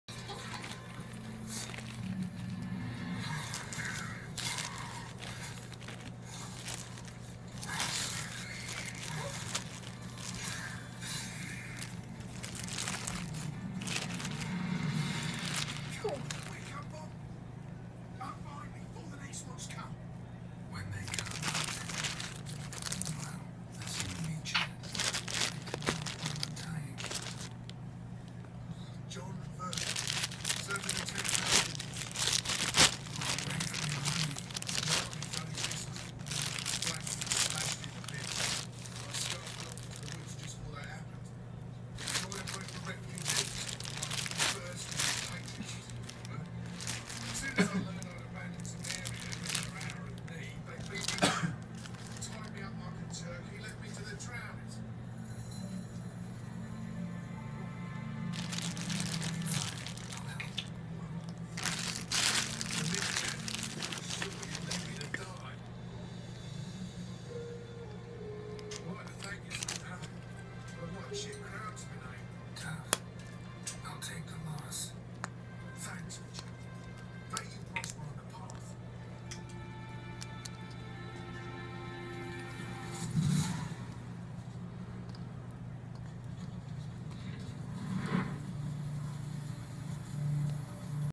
The bag is way more fun than the toys Mom!
Gotcha the Moluccan Cockatoo.
Follow Gotcha on Instagram! As always the bag or the box that new toys come in is always more fun than the toys themselves! ;) lol